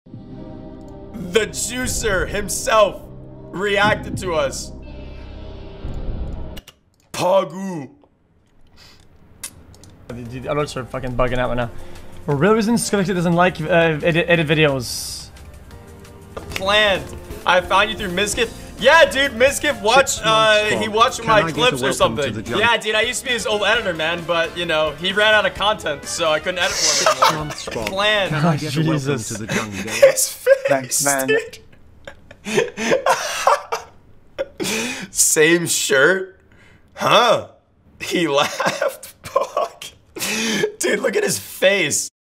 xQc reacts to XQC CLONE
Clip from one of Luke's streams.
Idea and clip + editing help by Squidward. I appreciate you very much, buddy. ♥